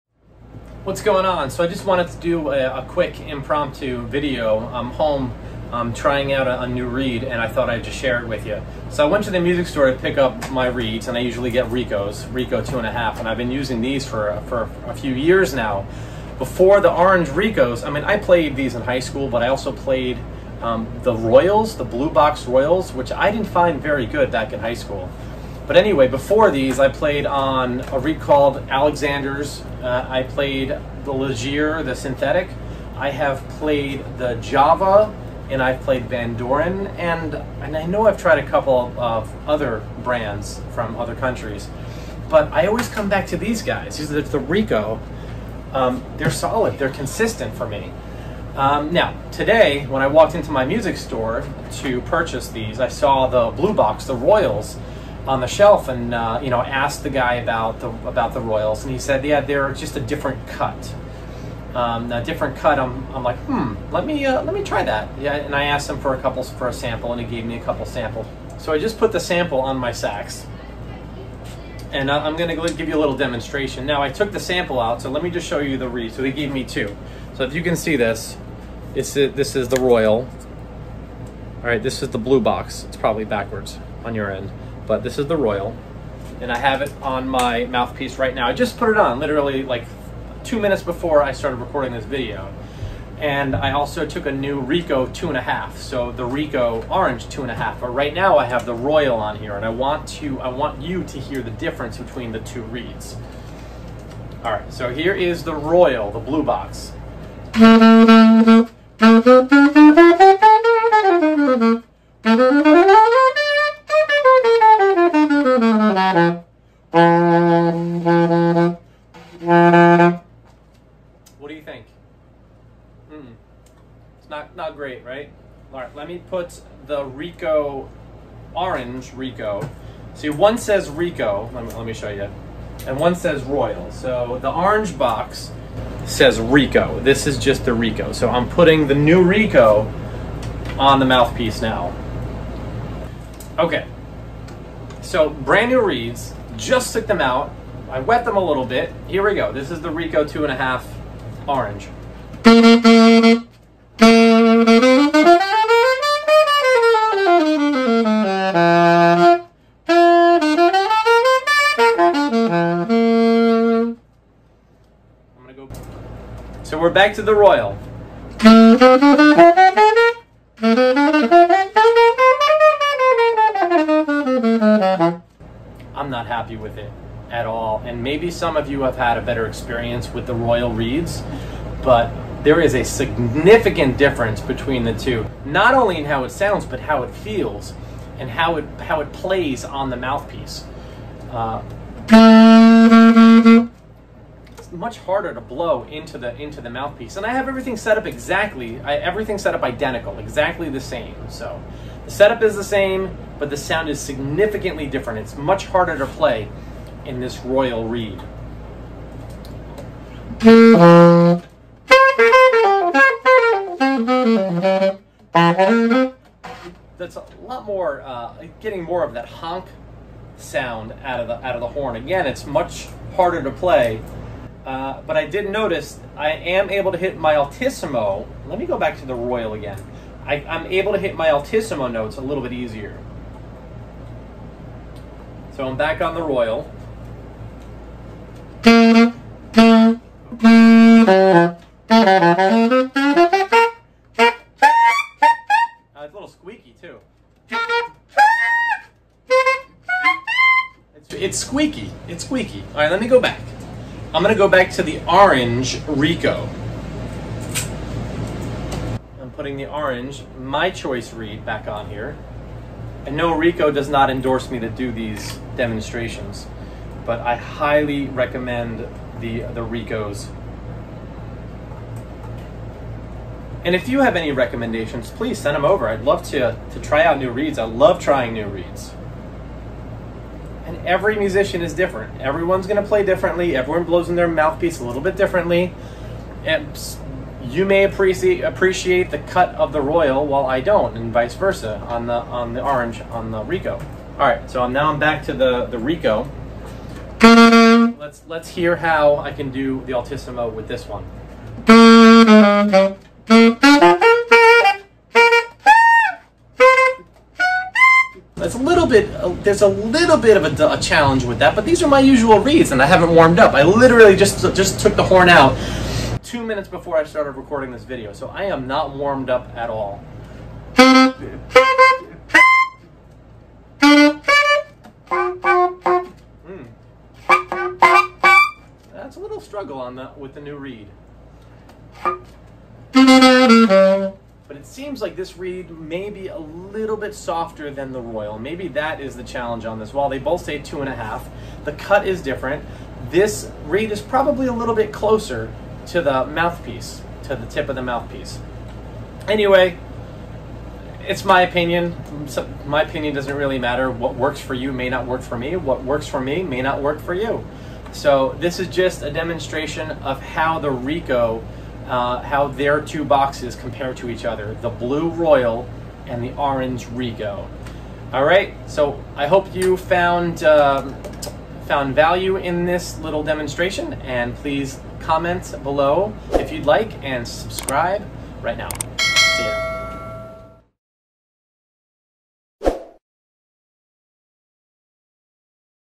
Rico Reed DEMO: Orange Vs. Blue
The Orange Box Rico Reeds Vs. The Blue Box Rico Reed.  Different cuts, different feels.  I can't stand one of them.

Thanks!